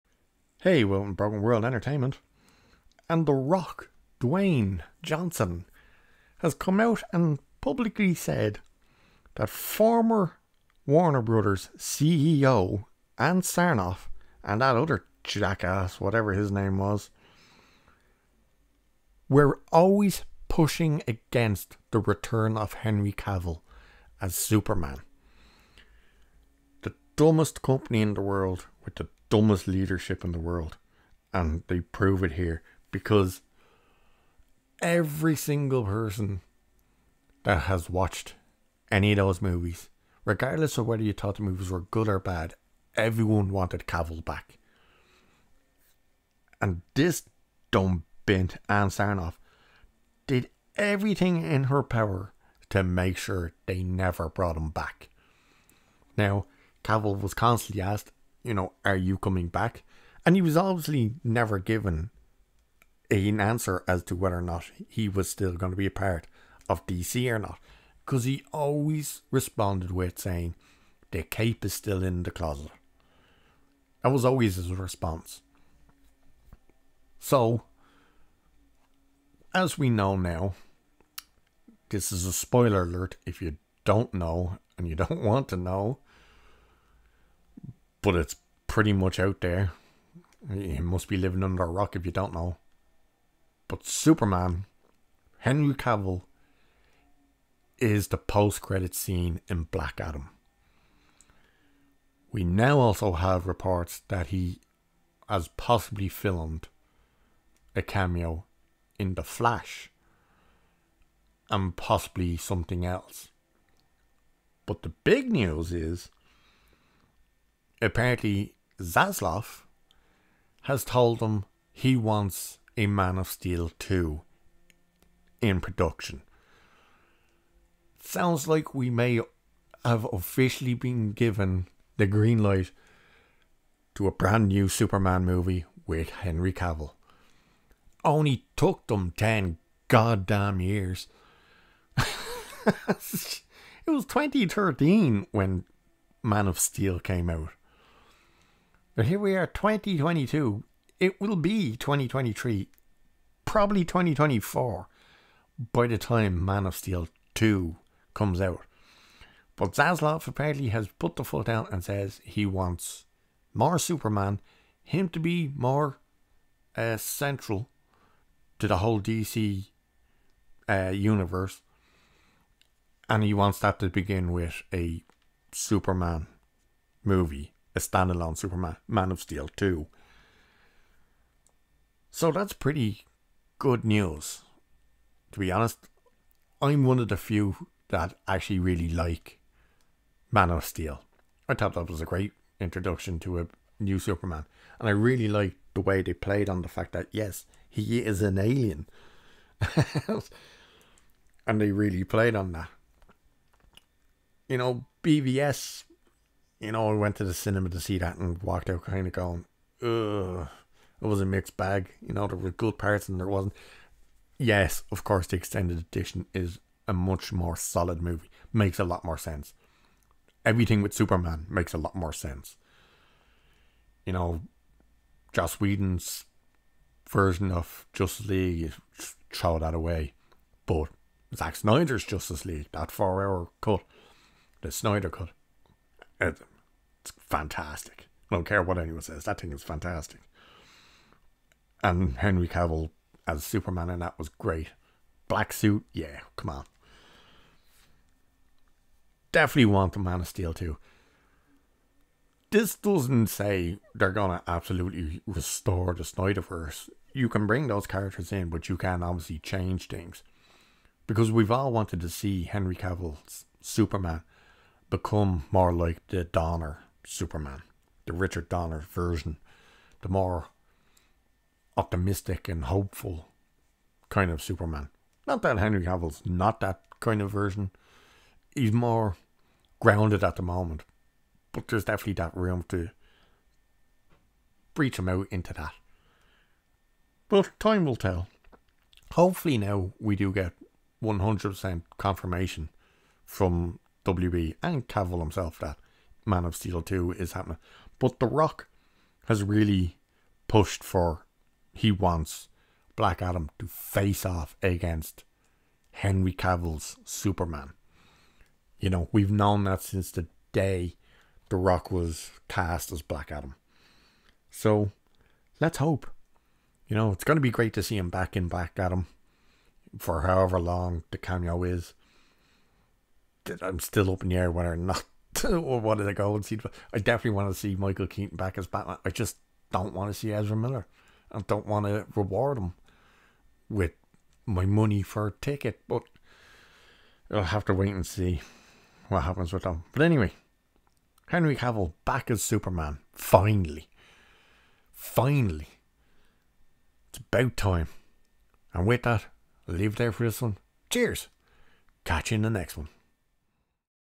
Superman Is Back
Henry Cavill is set to return with Man Of Steel 2